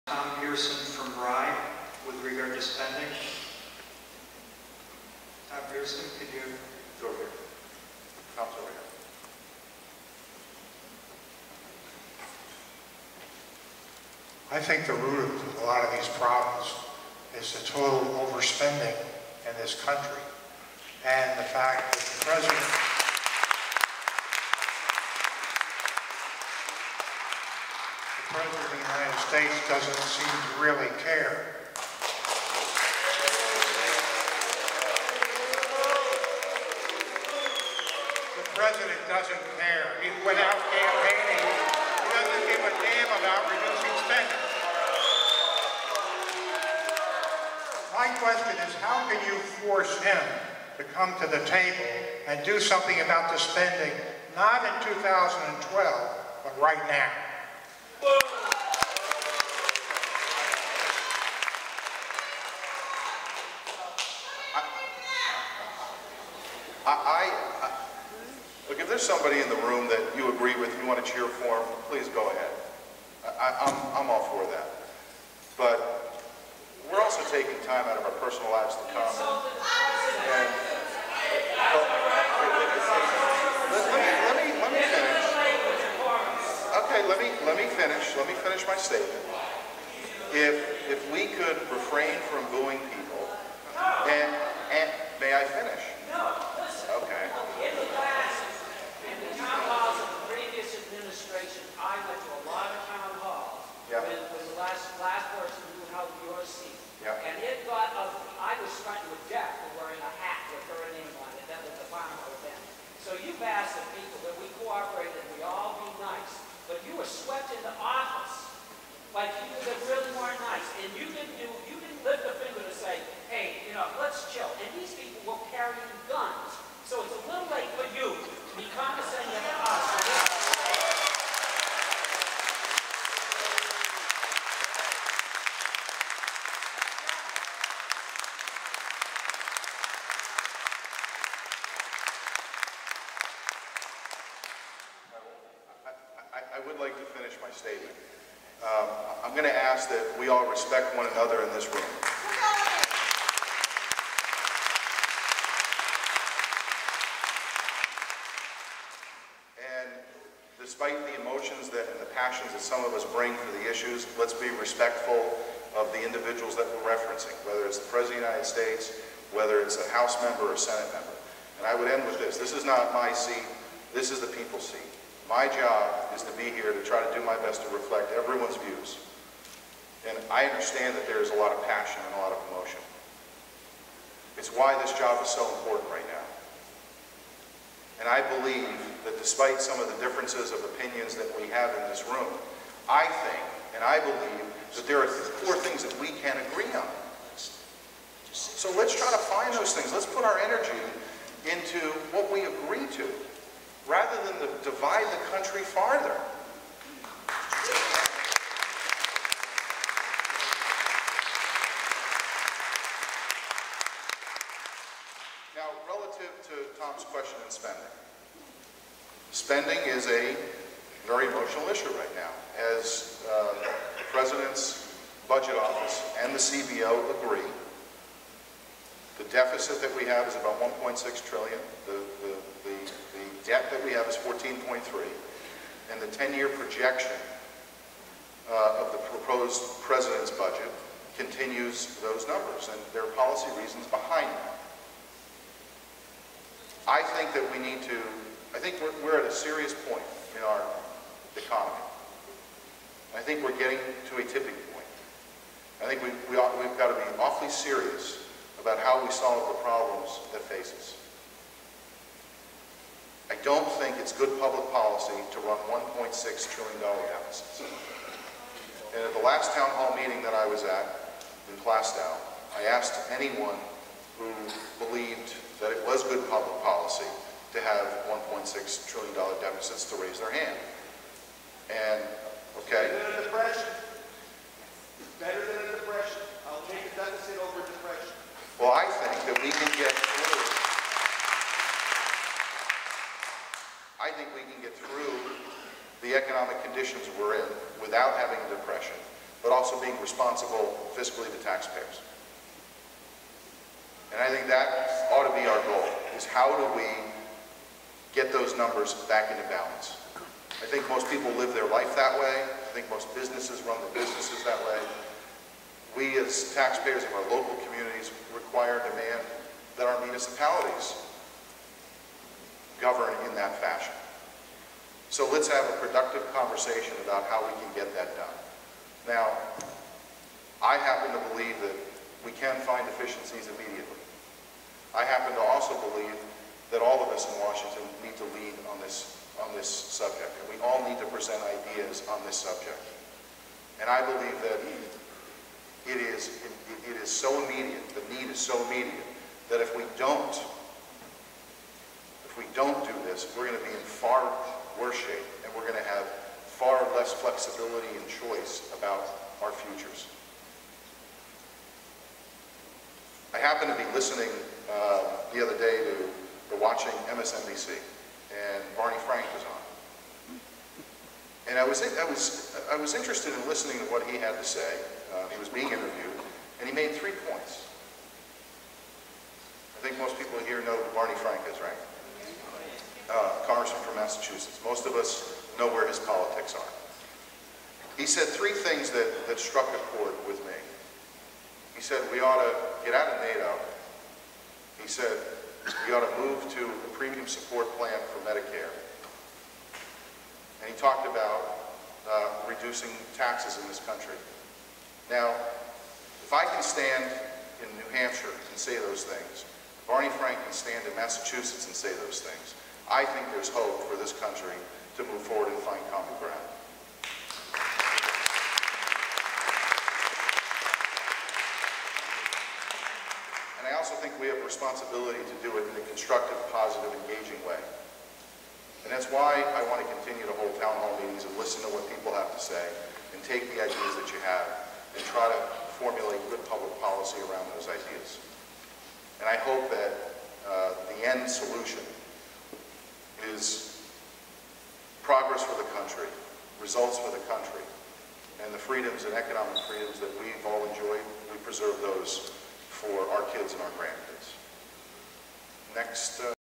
Congressman, Frank Guinta at Exeter Town Hall Meeting Meets Significant Opposition
Congressman, Frank Guinta at Exeter Town Hall Meeting Meets Significant Opposition to his vote for the Paul Ryan budget. Seacoast New Hampshire residents expressed that they do not want Medicare and Medicaid trashed, and they want tax cuts for the wealthiest Americans to be ended out of fairness. Raymond resident Chaz Proulx chastised Guinta for complaining about some angry citizen comments coming from the audience when he was swept into office by very angry, gun-toting Tea Partiers who mercilessly attacked his predecessor, former Congresswoman Carol Shea-Porter as he said nothing about proper decorum then.